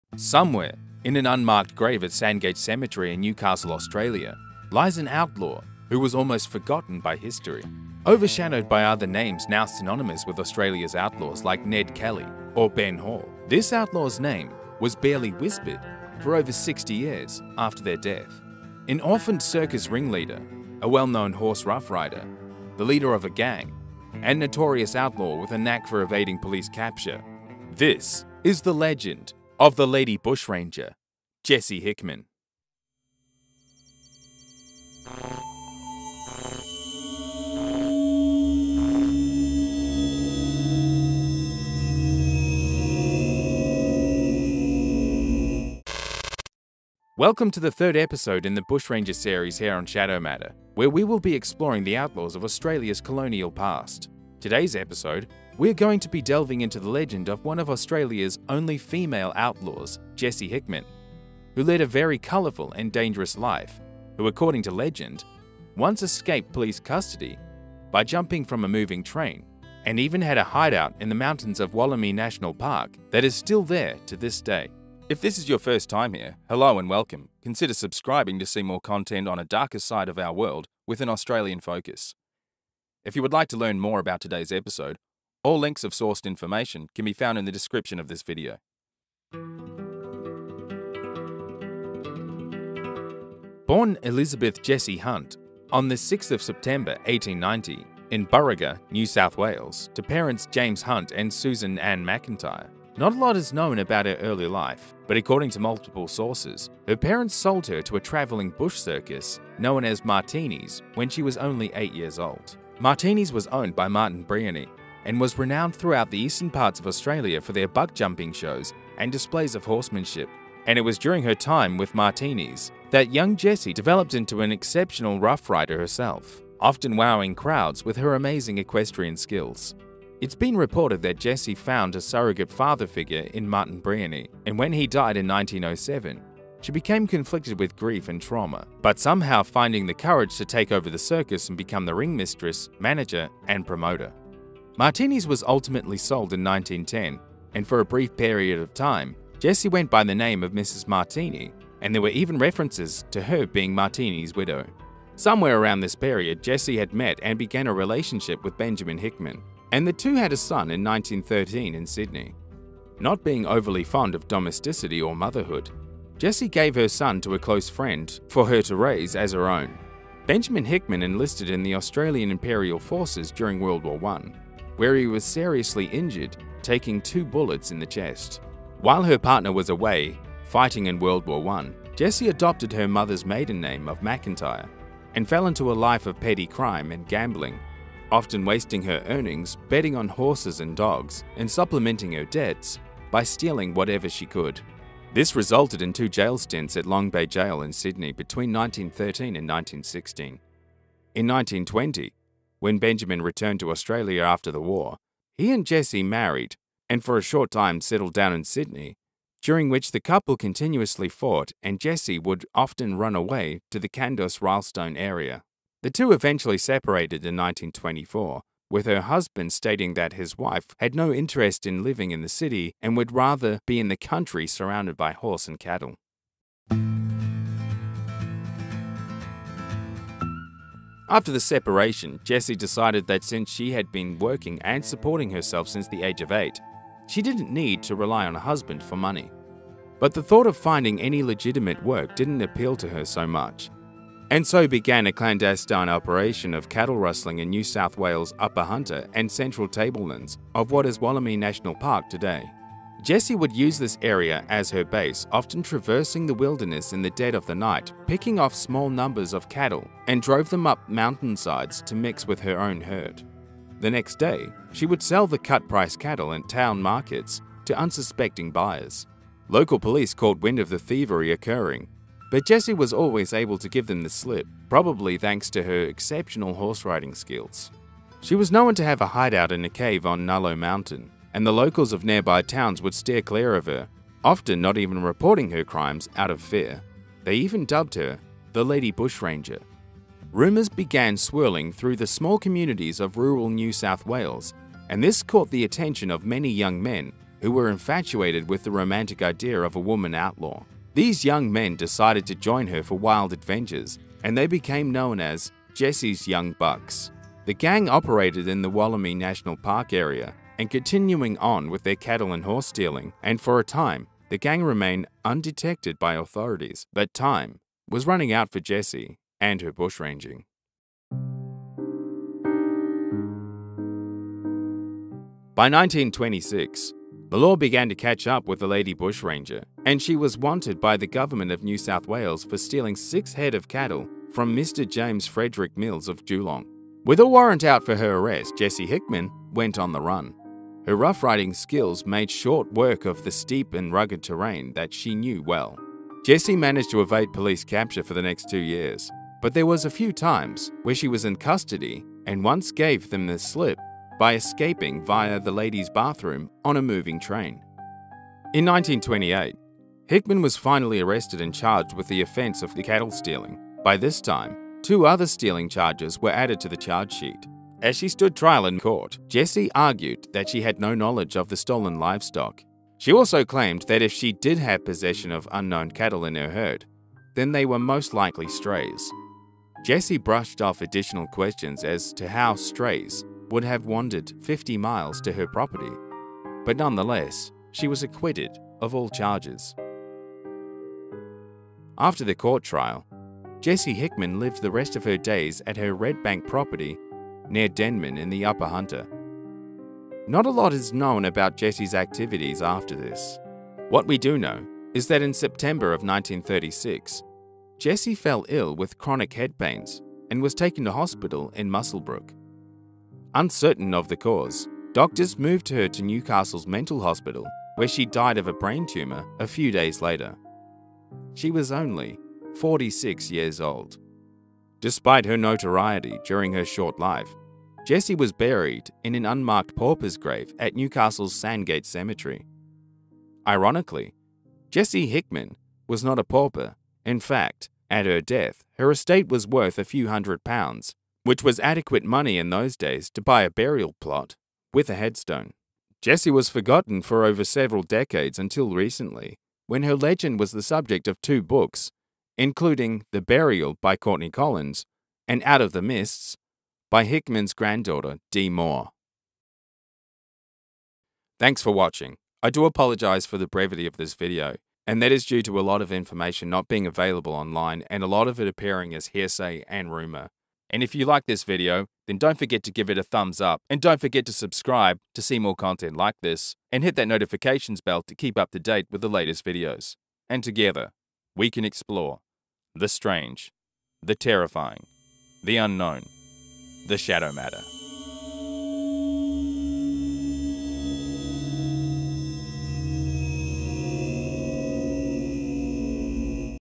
The Lady Bushranger | Jessie Hickman
The Legend of the LADY BUSHRANGER: Jessie Hickman, was raised in the travelling bush circus Martini's in the early 1900's before becoming one of Australia's almost forgotten outlaws,
Legend has it that she escaped from a moving train while being in custody of the NSW POLICE.
Escaping to the Wollemi National Park area, JESSIE HICKMAN, evaded police capture for over two years while running a gang of young men and cattle rustlers.

Australia, Australian Outlaws, outlaws, Martini's bush circus, Shadow Matter,
australian women outlaws